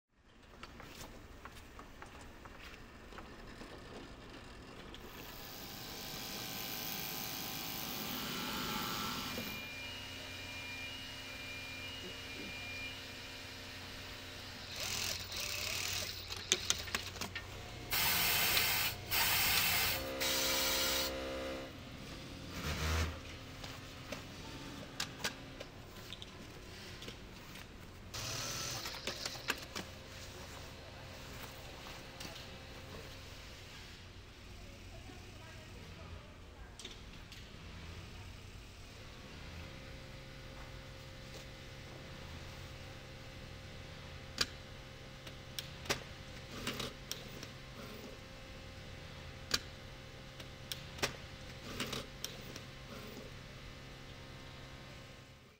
Arc'teryx Medical Gowns
From Alpha SV to PPE. In collaboration with local health authorities, we've created a reusable medical gown to address the current shortage as a result of Covid-19.